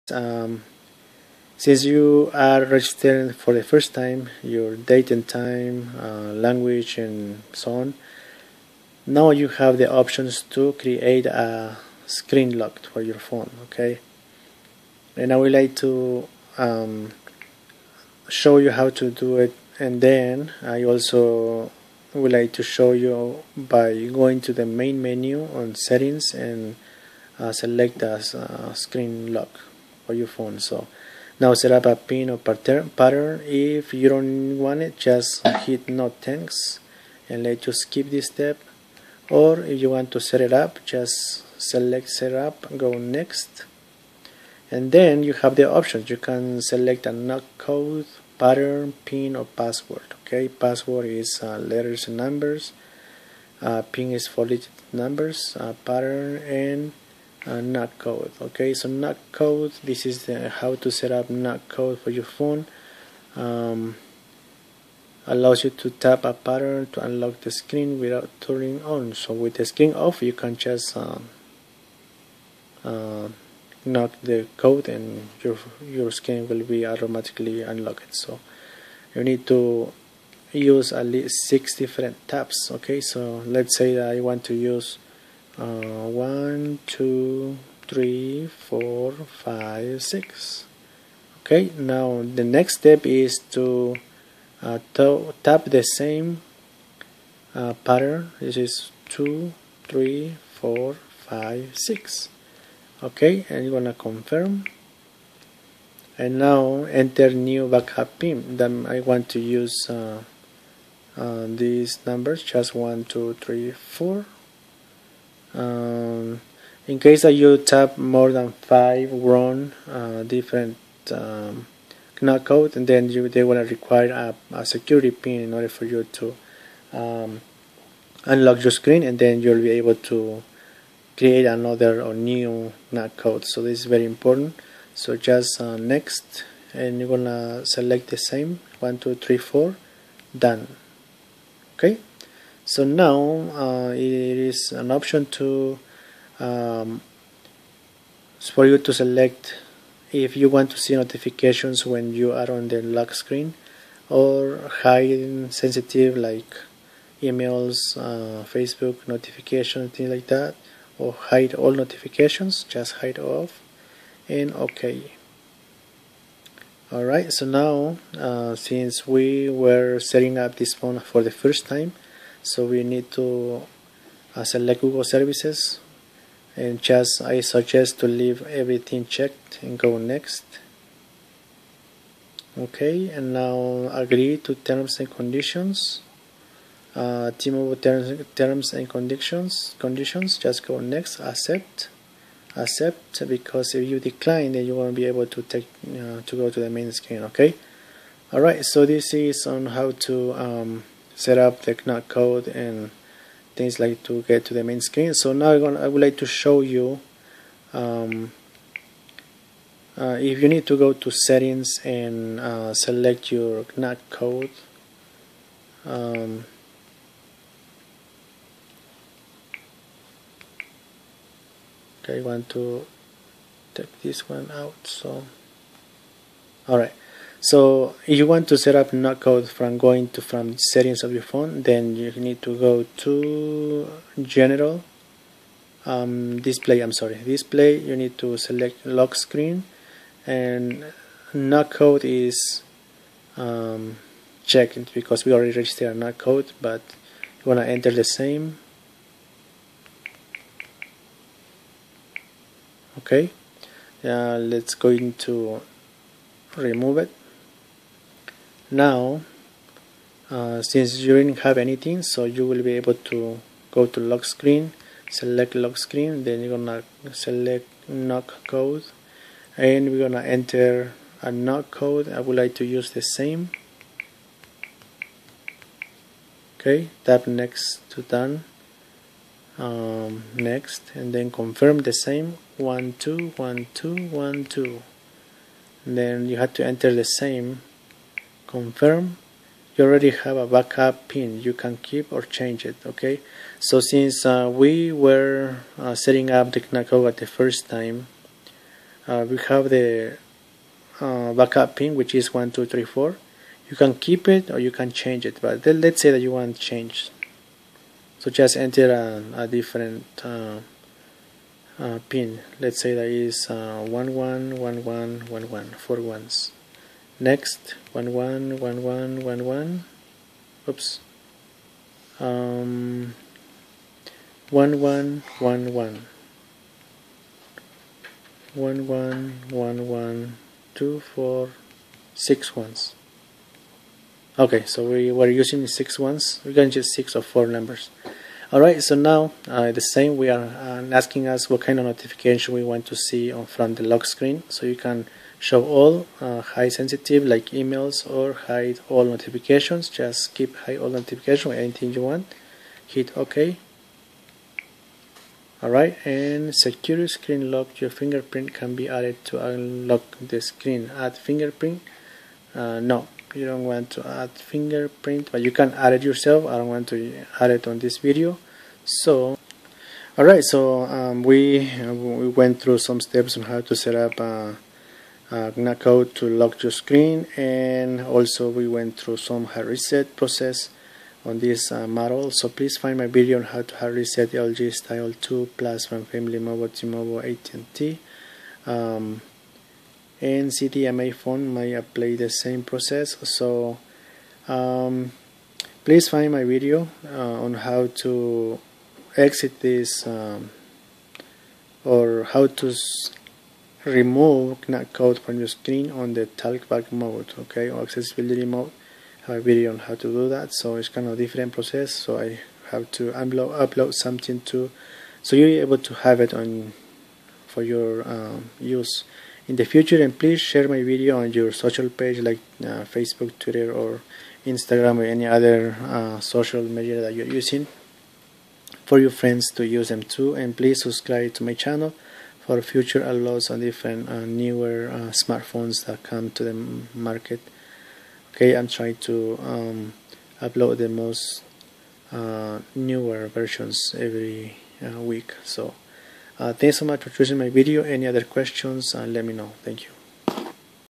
How to set knock code as lock screen on LG Stylo 2 plus
This video will guide you through steps to create knock code for your lock screen. We are using the LG model K550 in this video